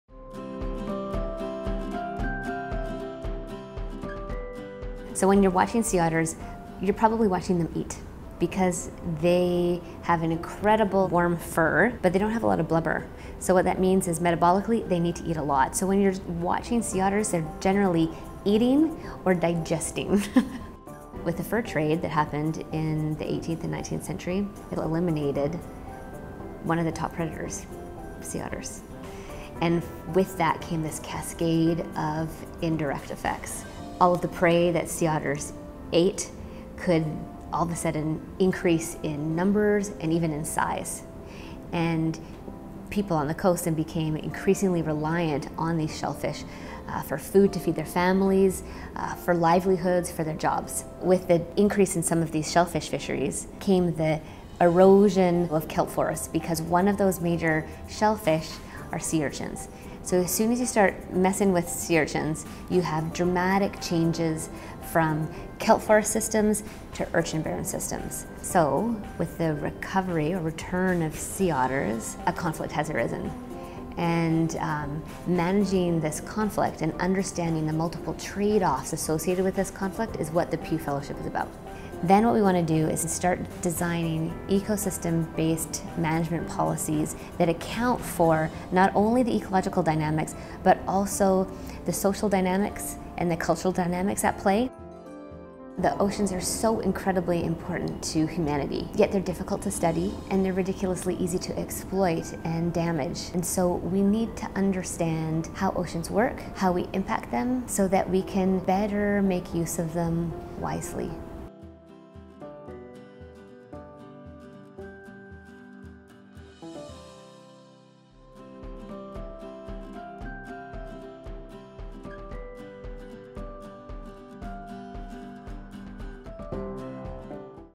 Sea Otters vs. Urchins in Canada's Kelp Forests | One Minute Dive with Pew
“When you see a sea otter, they’re usually either eating or digesting,” often munching on urchins, says ecologist Anne Salomon, a Pew marine fellow. That's a good thing for some kelp beds. Without otters to control urchin numbers, the spiky shellfish can devour the beds, leaving barren seascapes behind.

Fifty years ago, sea otters were so sought after for their fur that they disappeared from the Canadian coast. But now they're bouncing back and—as seen in this video—competing with humans for the region's shellfish.